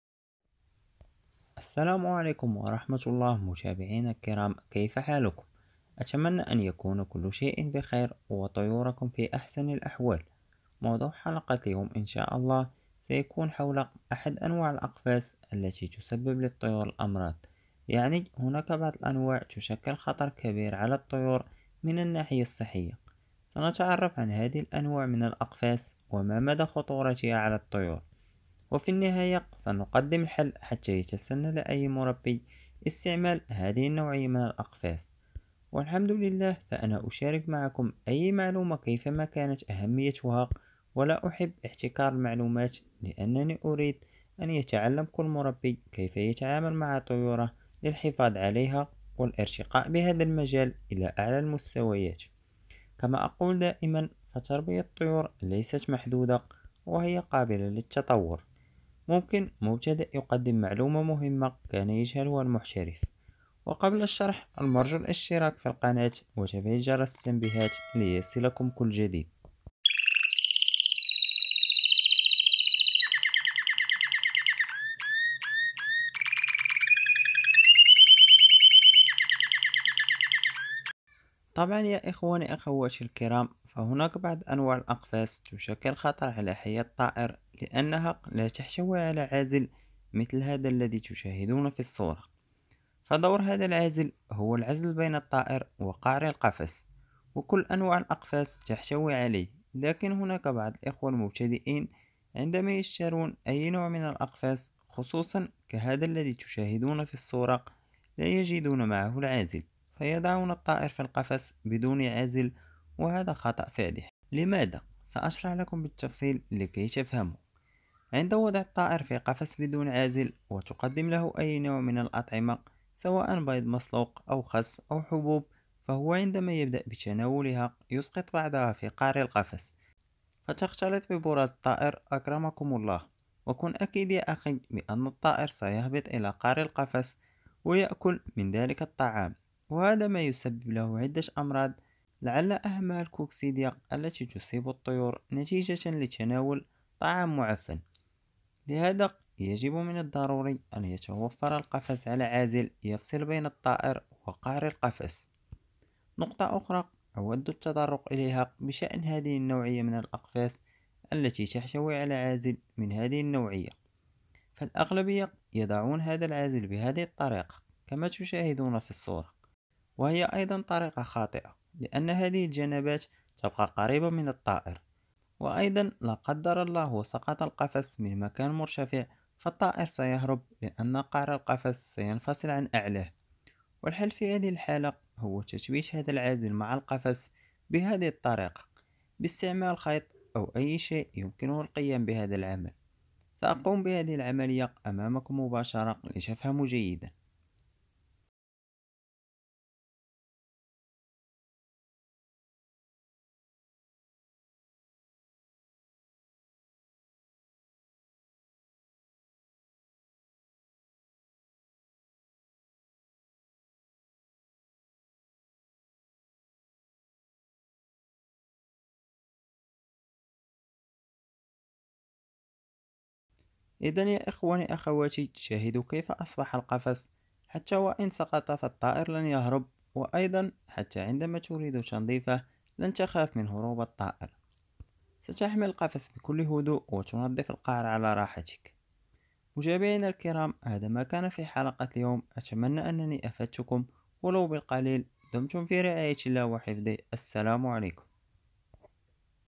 هذه النوعية من الأقفاص ممكن ان تكون سبب في مرض طيورك ⚠️⚠️
🦜🦜🦜  صفحة  الفيسبوك
مجموعة الفيسبوك 🦜🦜🦜

كيفية تربية طيور الكناري (نصائح_علاج_وقاية)

تجارب على طيور الزينة 🦜🦜